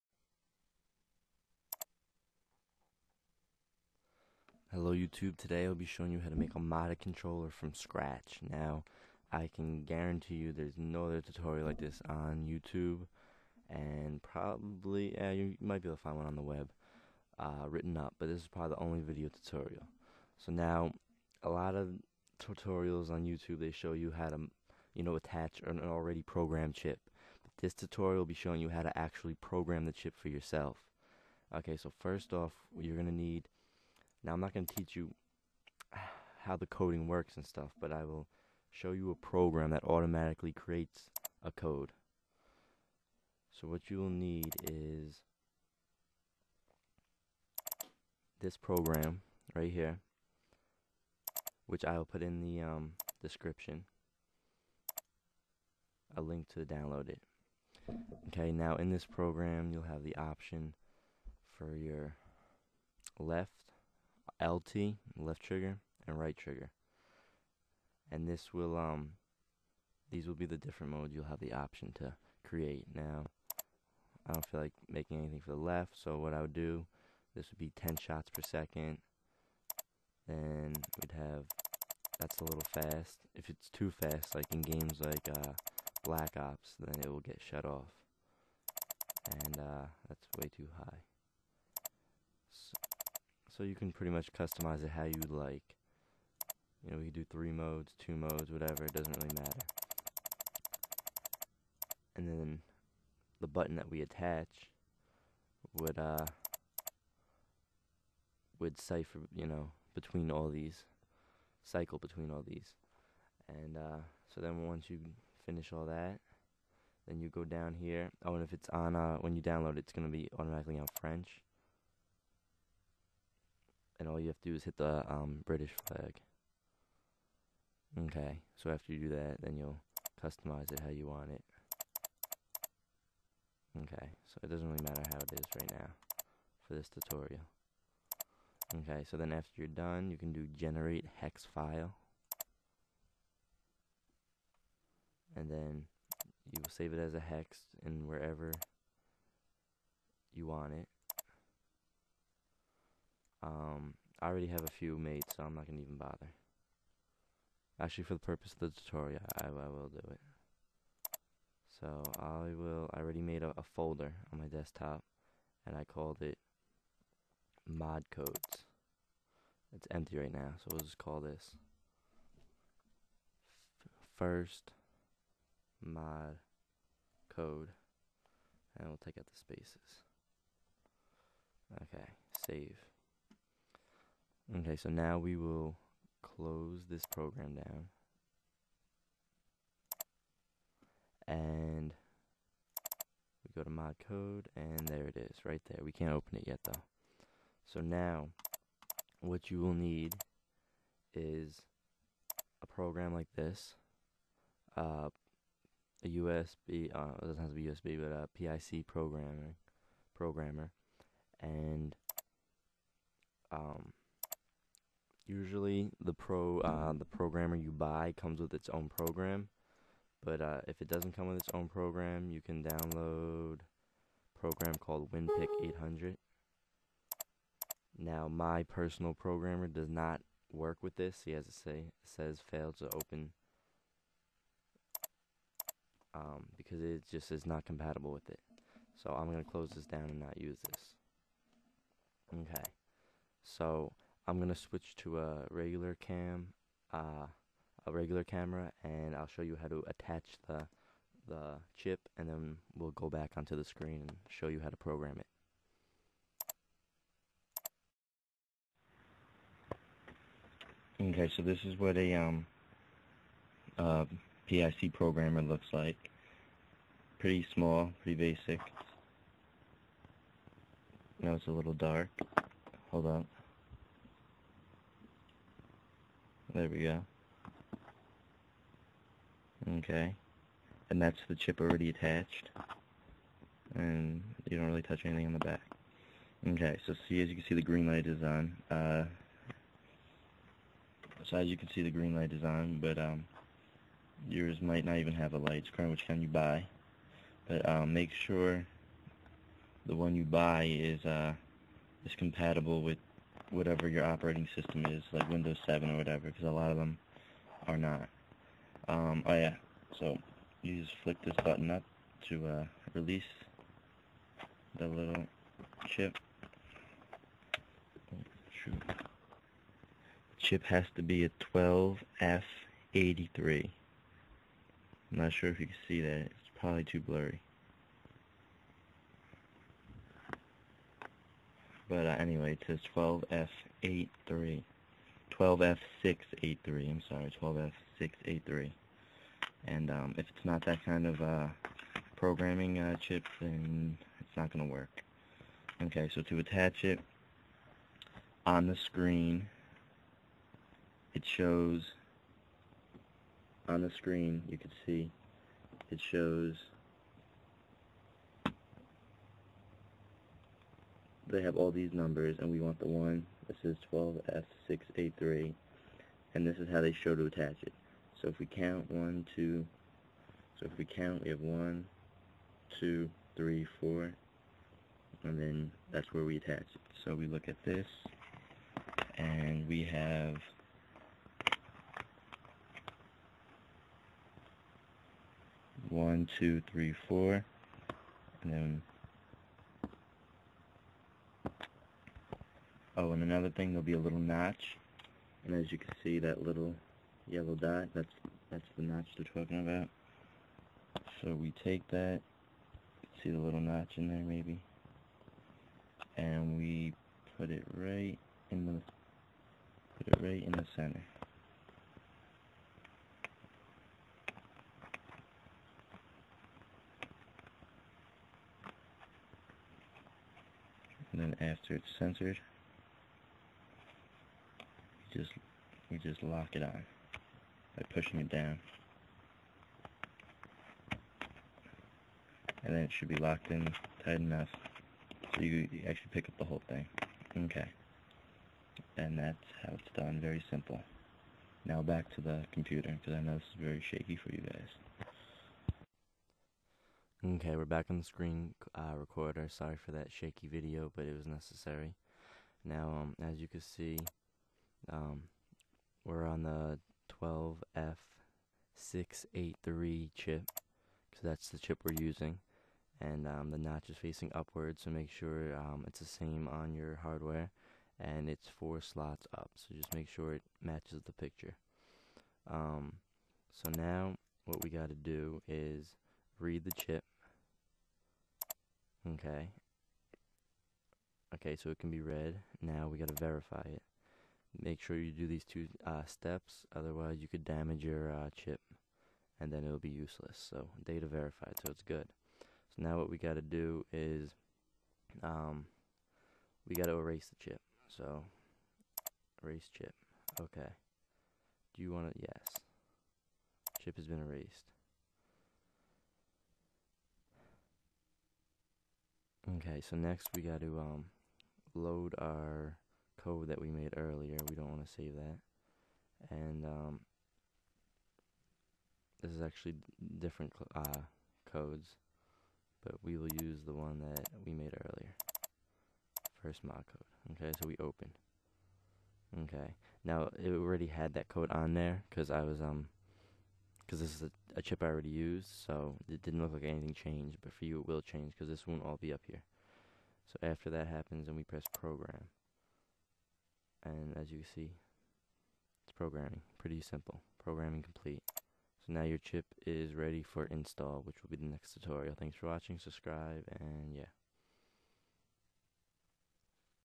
How to make a Rapid Fire Mod Controller(part 1 - programming the chip)
This video will show you how to program the PIC chip that you will be needing for install on an Xbox 360 controller.

Software needed:
PIC Programmimg software(ex. "WinPic 800")(usually comes with the programmer you purchase)

Code Generator (click the blue button to download, not the green one)

Hardware needed:

PIC Programmer Microchip 12F683(if u have win7, make sure its compatible)(Mine cost $25 off of eBay.)